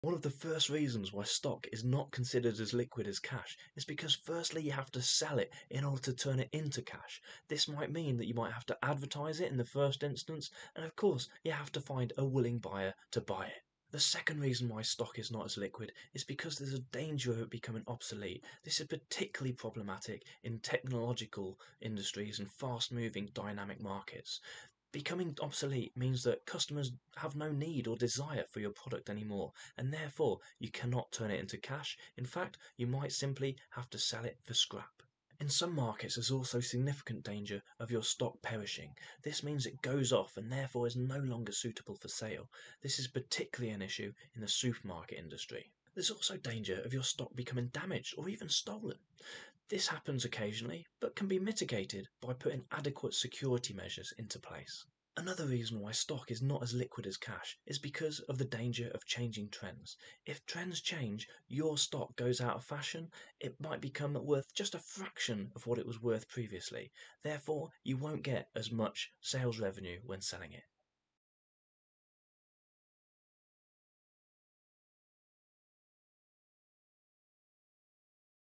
Why is STOCK not as LIQUID as CASH?
Stock is not considered as liquid as cash and therefore cannot be used as easily to pay off debts that the business owes (current liabilities). There are a number of reasons why stock (otherwise known as inventory) is not as liquid as cash and this video looks at each of these in depth.

Video chapters:
0:00 Businesses must sell it first
0:14 Danger of becoming obsolete
0:35 Danger of perishing
0:45 Danger of damage
0:55 Danger of change in trends